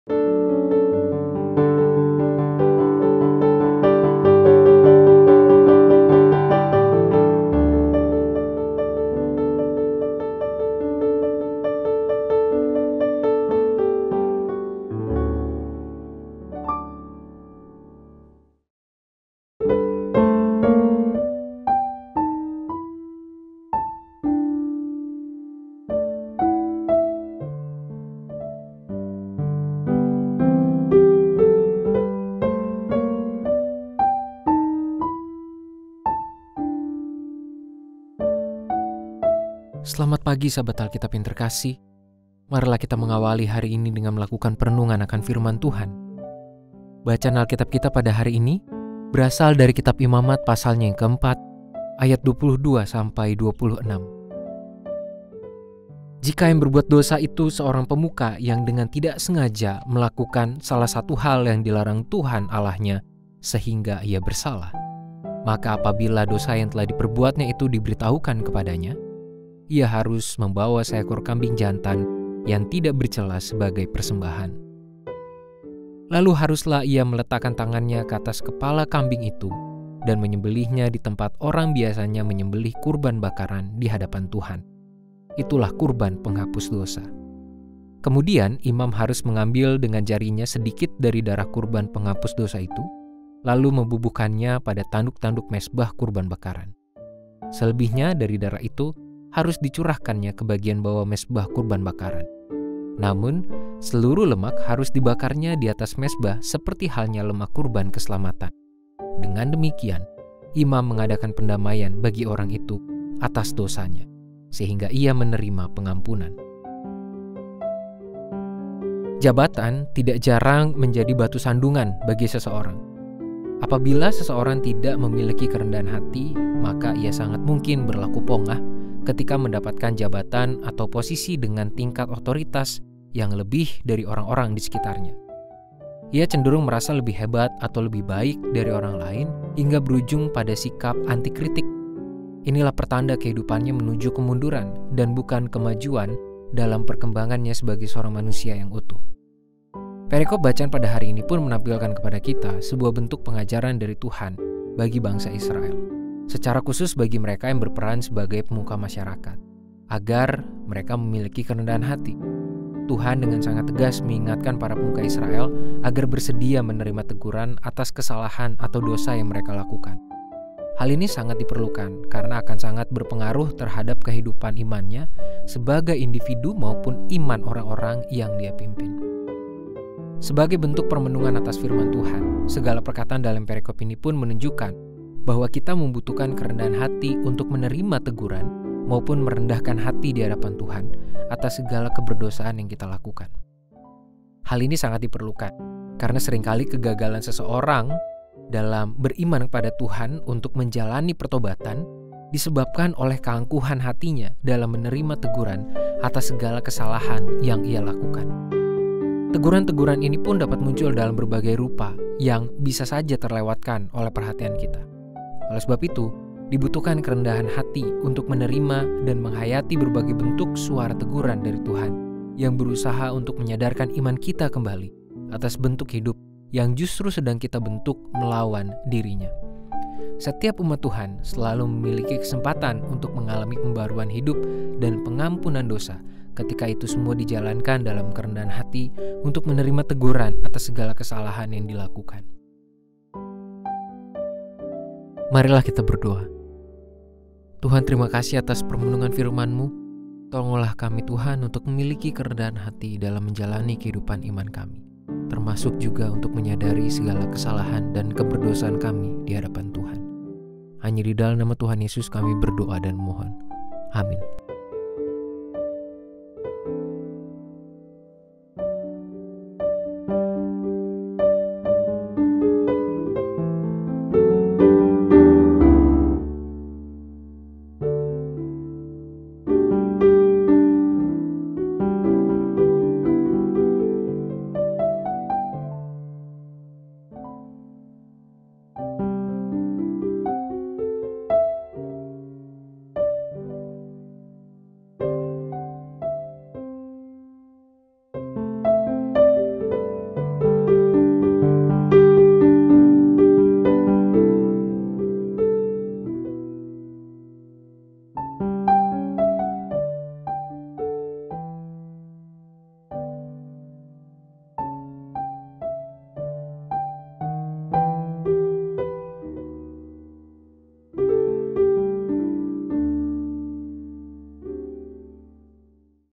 MENGAKU DOSA DENGAN KERENDAHAN HATI - Daily Scripture Reading 9 Maret 2023
Bacaan renungan: Imamat 4:22-26 (TB2)

Setiap umat TUHAN selalu memiliki kesempatan untuk mengalami pembaharuan hidup dan pengampuan dosa ketika itu semua dijalankan dalam kerendahan hati untuk menerima teguran atas segala kesalahan yang dilakukan.